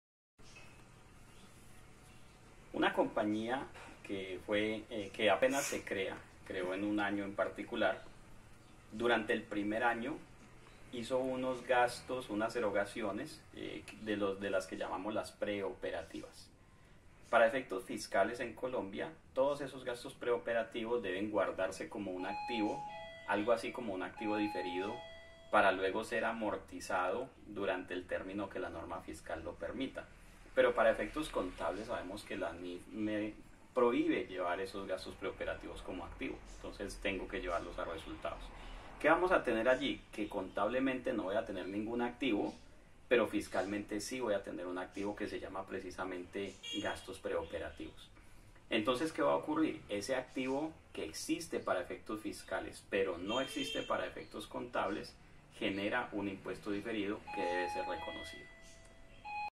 Los gastos preoperativos reconocidos como activo para efectos fiscales generan impuesto diferido
Un empresa recién constituida incurrió en gastos preoperativos por 70 millones. Estos gastos de reconocen como activo para efectos fiscales, pero se llevan a resultados en los estados financieros. Dado que más adelante afectarán el cálculo del impuesto a las ganancias, debe reconocerse un activo por impuesto diferido